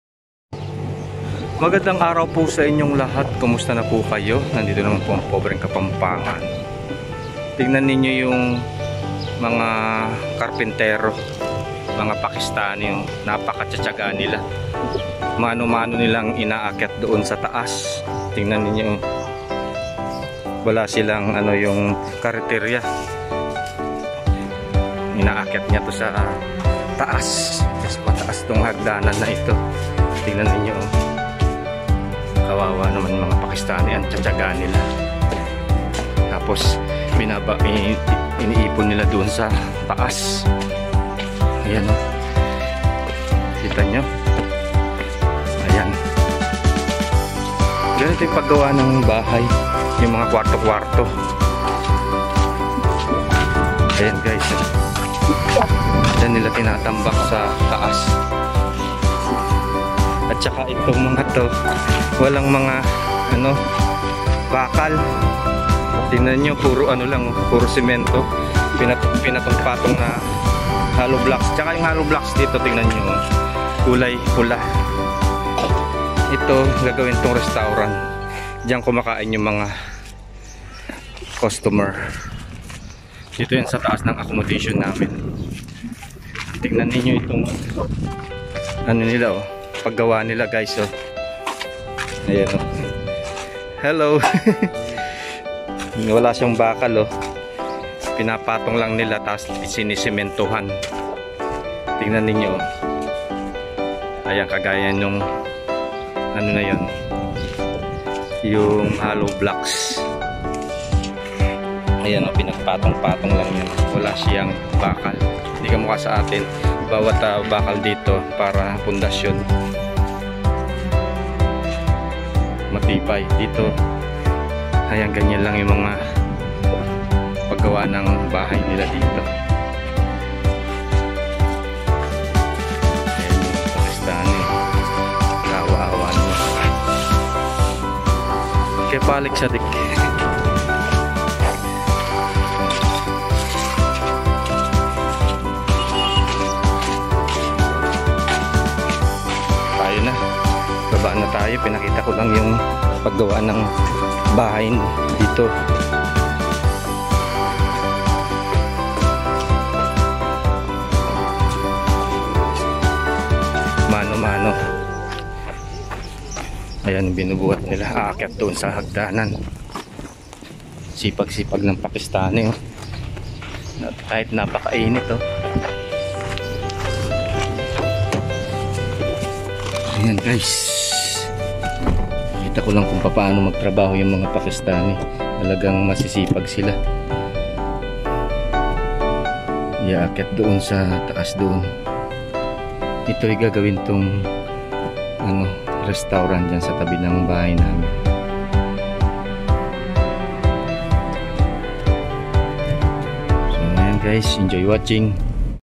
PAKISTAN CONSTRUCTION WORKERS PAANO ANG PAGGAWA NG BAHAY DITO SA SAUDI ARABIA
Ipapakita ko sa inyo itong trabaho ng construction

Construction WORKERS HERE IN SAUDI ARABIA

Pakistan construction worker

How a build house
By manual hand carry soil going up